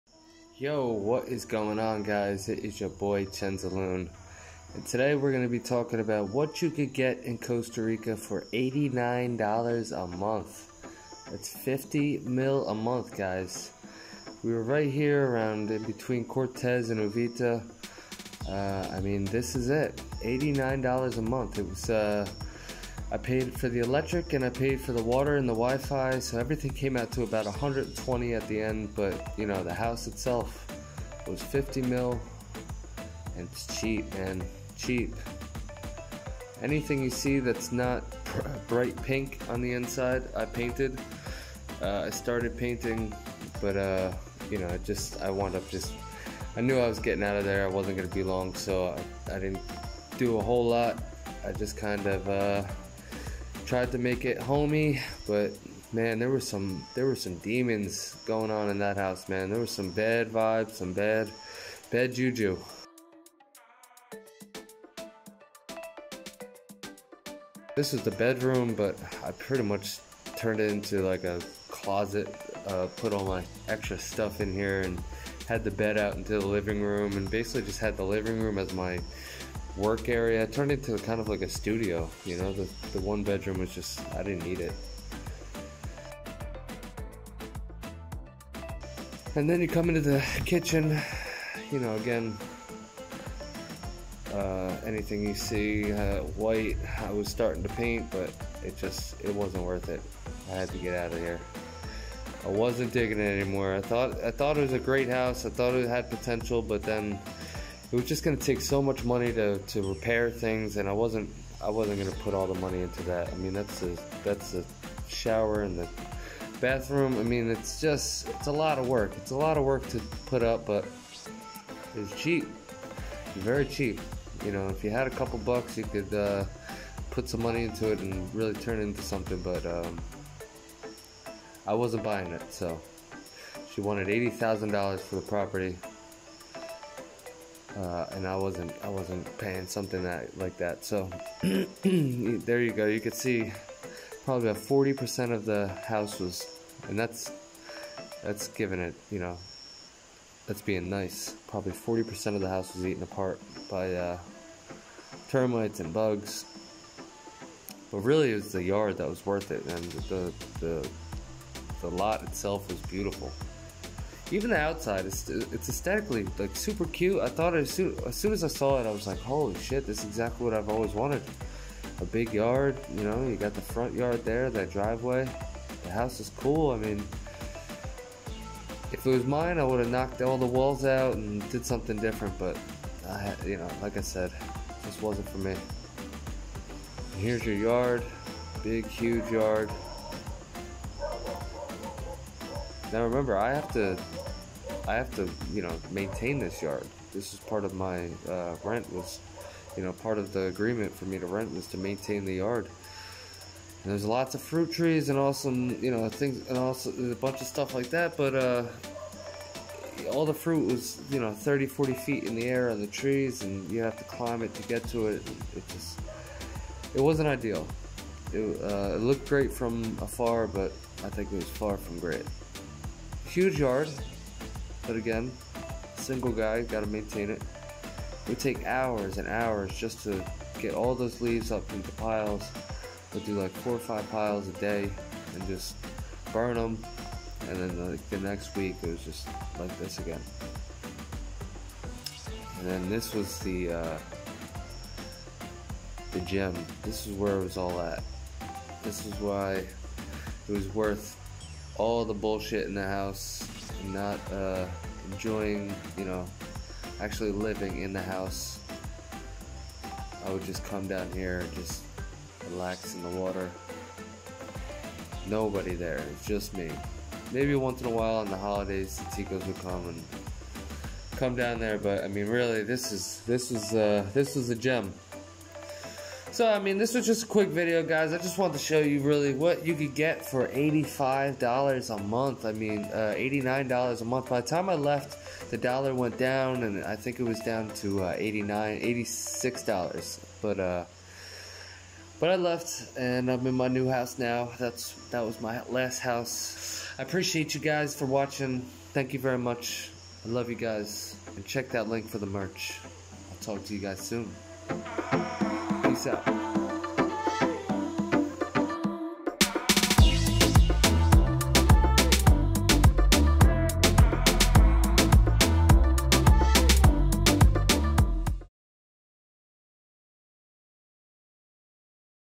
$89 RENT - Costa Rica
I lived in this house for a little over a year. Not the most beautiful place I've stayed, actually probably one of the worst, but the cheapest by far!

MERCHH!! -  chenzaloon.company.site